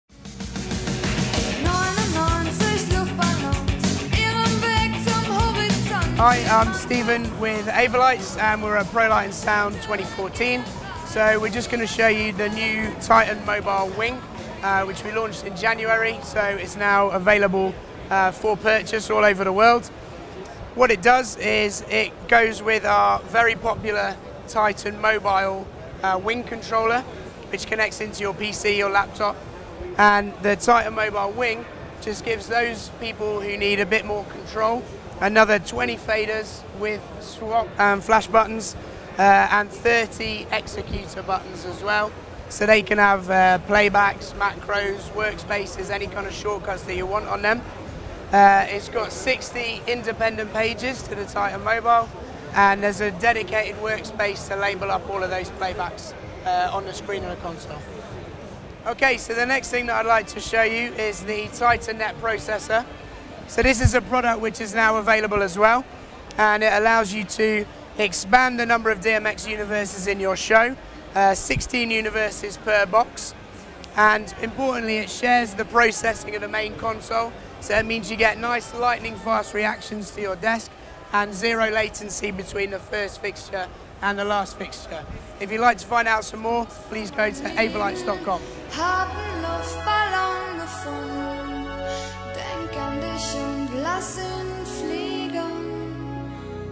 PLS2014: Avolite Titan Mobile Wing & Titan Net Processor
Stephen Baird-Smith from Avolites introduces the latest in controls. The Titan Mobile Wing and the Titan Net Processor at ProLight+Sound 2014.